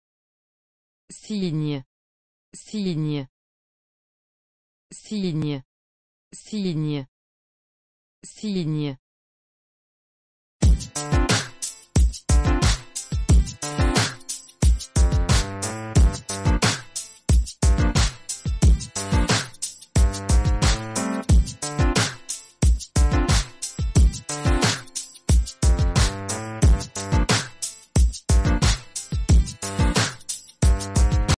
How do you say 'sign' in French?
How to say 'sign' in French? Listen to hear the pronunciation.